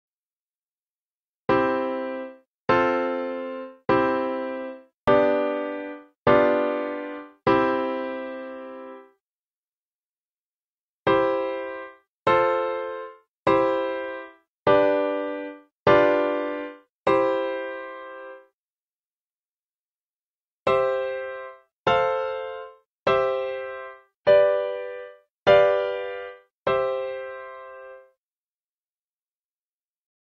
C Cadence Inversions, Right Hand: I-IV-I-V-V7-I
This demonstrates with the right hand the basic cadence I-IV-I-V-V7-I in three positions: root, first inversion, and second inversion.